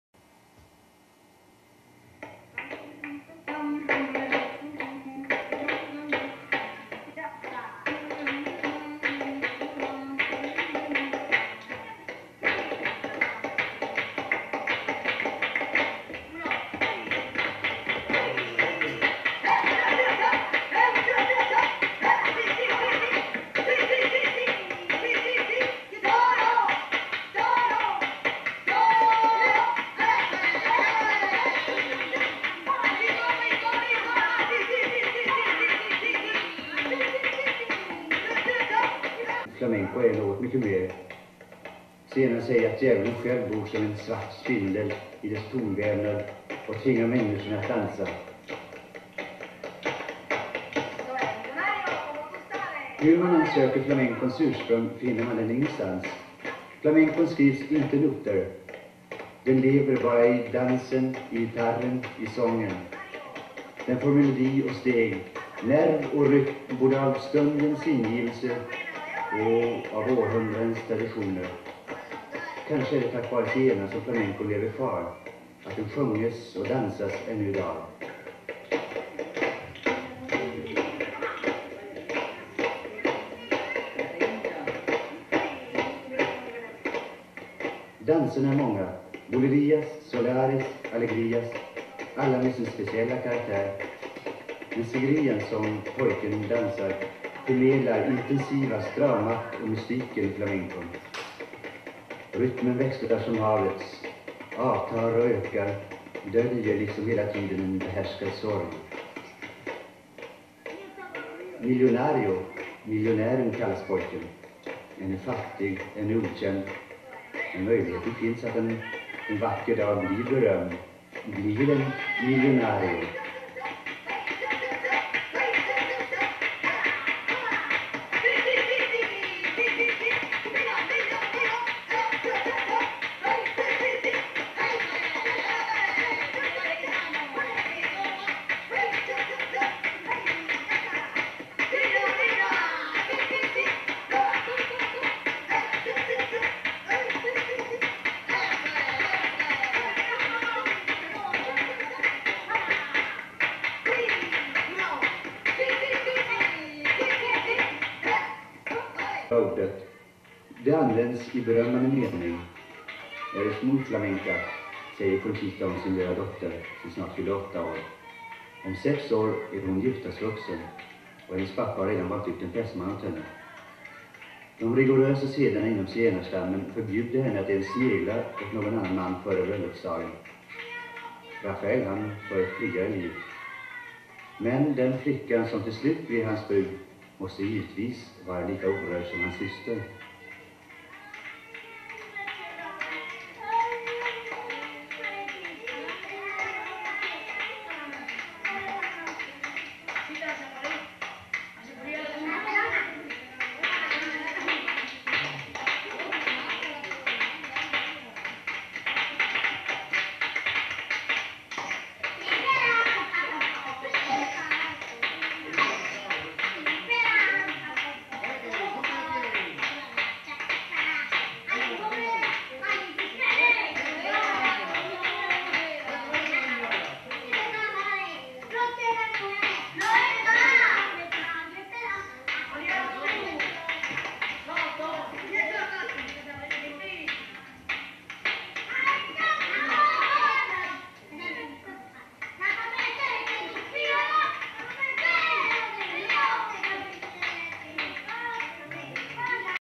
FLAMENCO Andalu' / На цигането му е в кръвта, в гена!
Borrowed with thanks. Pls waive (C) infringement for wider distribution. Rgds!
This video is part of a 1962 film about Gypsies.  This 12 year old boy dances first with no shoes and then in his full Flamenco attire.
Дванайсет-годишно циганче в Андалусия танцува ФЛАМЕНКО босоного отначало, а после в цялостен "костюм". В групичката виждаме други едвам  проходили, ала вече заредени и готови!